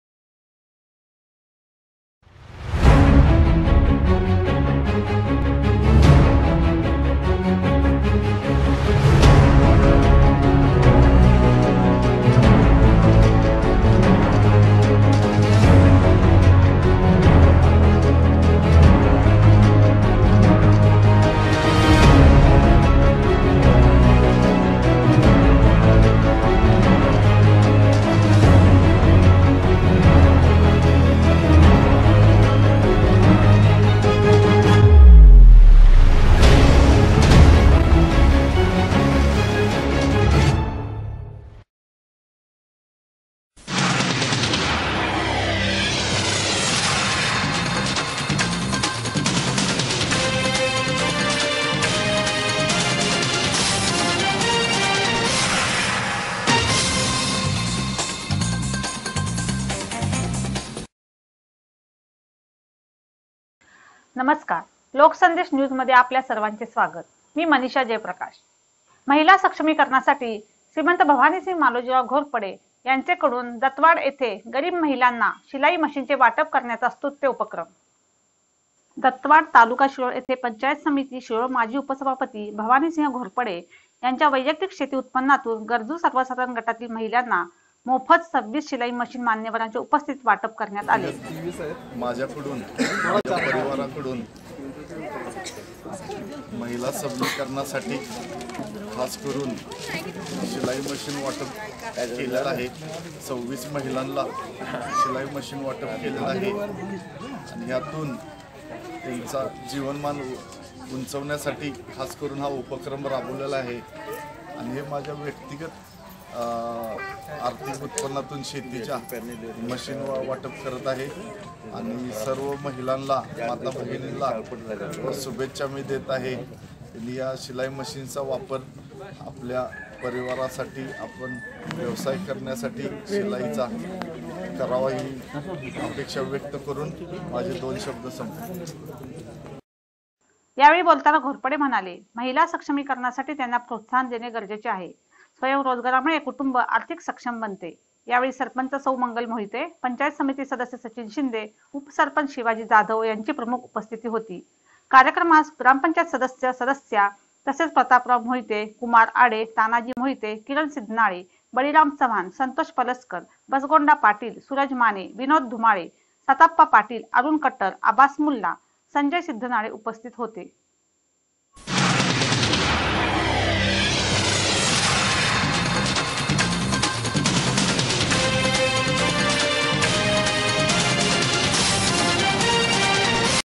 कोल्हापूर : शिरोळचे माजी उपसभापती भवानीसिंह घोरपडे यांच्या कडून महिलांना मोफत शिलाई मशिन वाटप.
कोल्हापूर : पंचायत समिती शिरोळचे माजी उपसभापती भवानीसिंह घोरपडे यांच्या वैयक्तिक शेती उत्पनातून गरजु सर्वसाधारण महिलांना मोफत शिलाई मशिन वाटप.